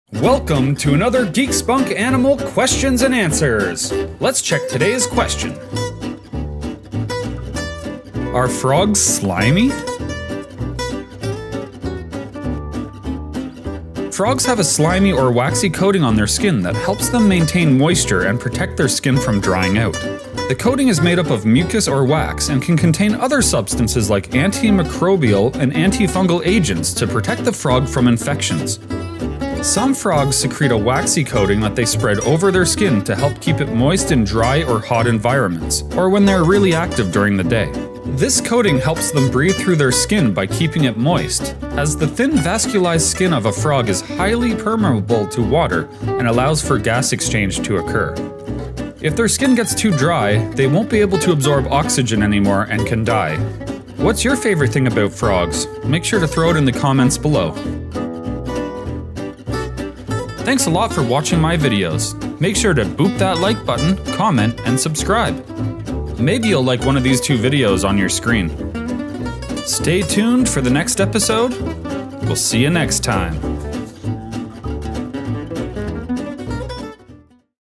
The Truth About Frog Slime: Are Frogs Slimy?
Frogs have a slimy or waxy coating on their skin that helps them maintain moisture and protects their skin from drying out. This coating is made up of mucus or wax and can contain other substances like antimicrobial or antifungal agents to protect the frog from infections.

Some frogs secrete a waxy coating that they spread over their skin to help keep it moist in dry or hot environments, or when they are active during the day.

This coating helps them breathe through their skin by keeping it moist, as the thin vascularized skin of a frog is highly permeable to water and allows for gas exchange to occur.

0:00 ► Intro